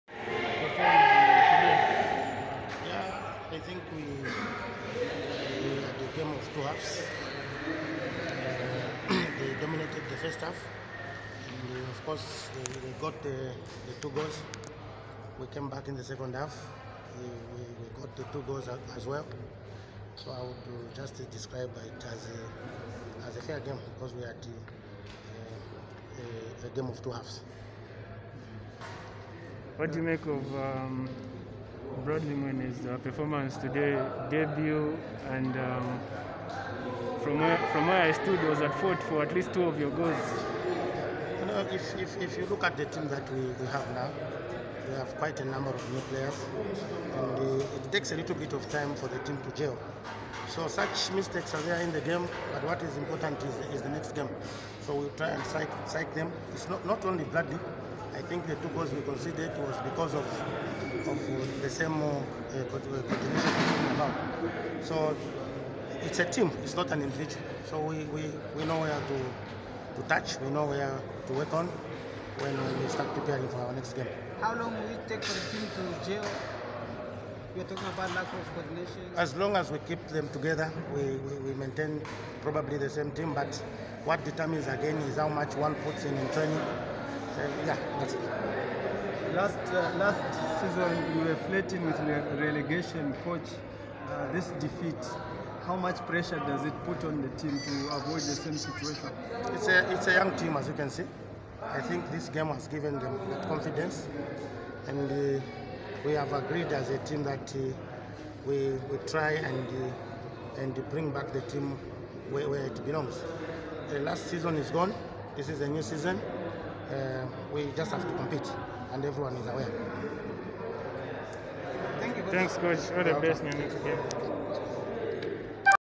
Nkana 1st Assistant Coach George Chilufya react to first 2021/2022 League defeat. Nkana 2-3 Nkwazi
Nkana 1st Assistant Coach George Chilufya react to the first 2021/2022 MTN Super League defeat. Nkana 2-3 Nkwazi